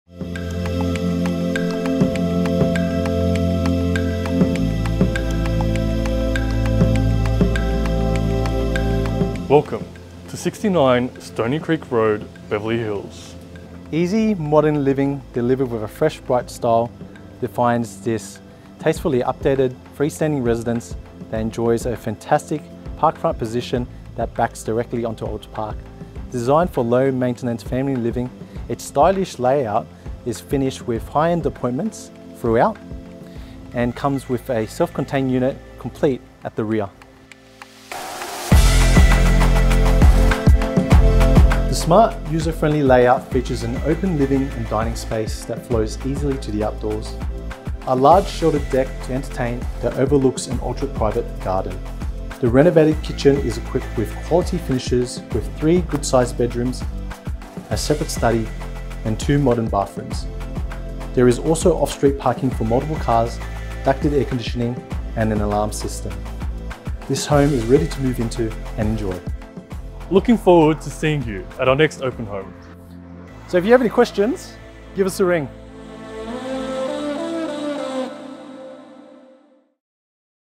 69 Stoney Creek Road, Beverly Hills
Easy modern living delivered with a fresh and bright style defines this tastefully updated  freestanding residence that enjoys a fantastic parkfront position backing directly onto
Olds Park.

Designed for low-maintenance family living, its smart and stylish floorplan is
finished with high-end appointments throughout and offers a layout that is great for
in/outdoor entertaining, plus it comes complete with a self-contained guest studio at the
rear.

The home sits on a well-sized level block and is very well-positioned close to a
range of schools, major shopping centres, bus transport, with acres of parkland and
green space just over the back fence.